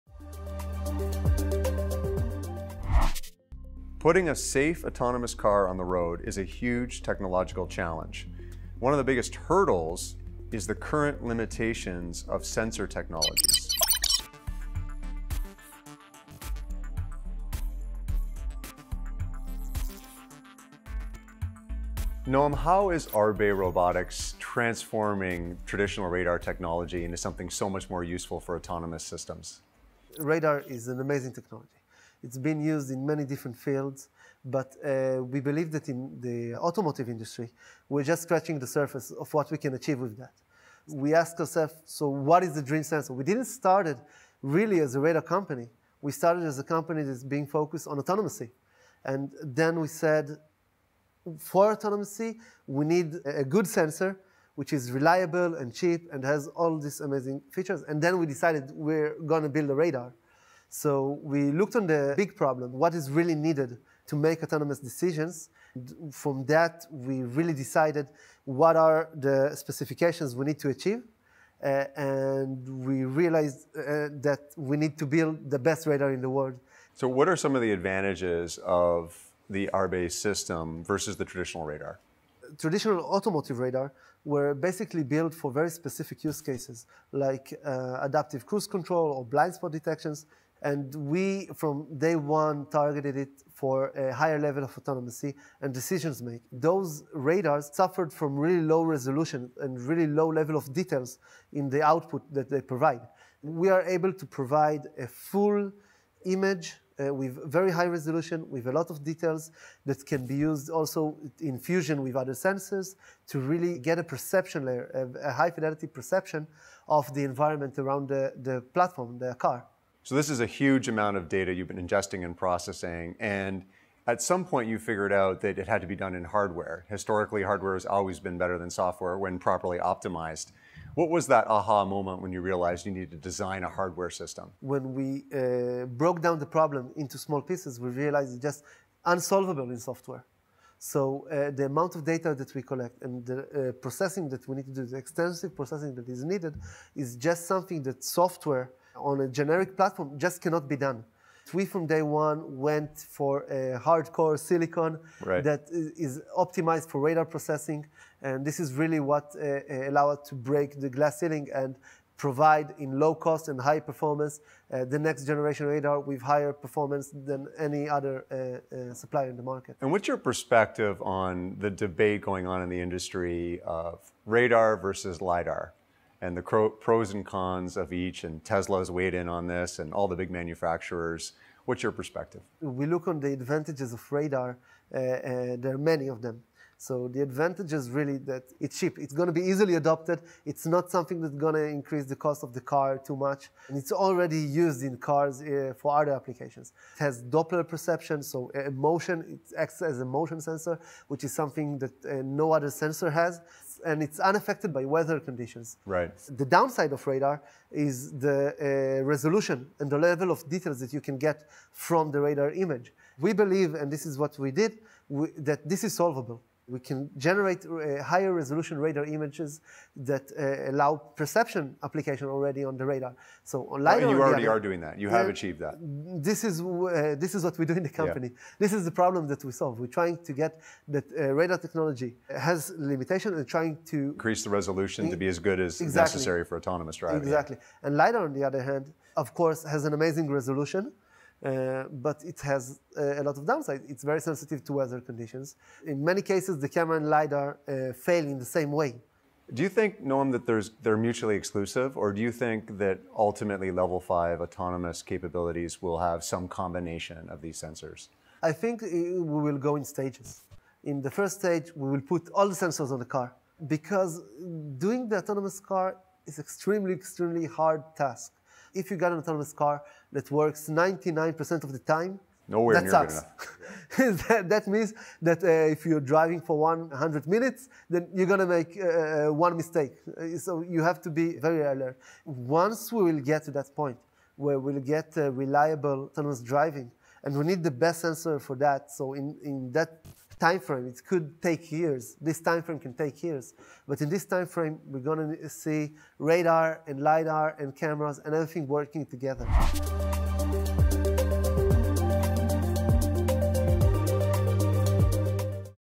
Beyond Innovation S3: Noam Arkind, Arbe Robotics
Safe, autonomous vehicles still pose a significant technological challenge due to the limitations of current sensor technologies.

Beyond Innovation's Anthony Lacavera sits down with Noam Arkind, Co-founder & CTO, Arbe Robotics, to discuss their high resolution, detailed image hardware that provides a high fidelity perception of the environment around an autonomous vehicle.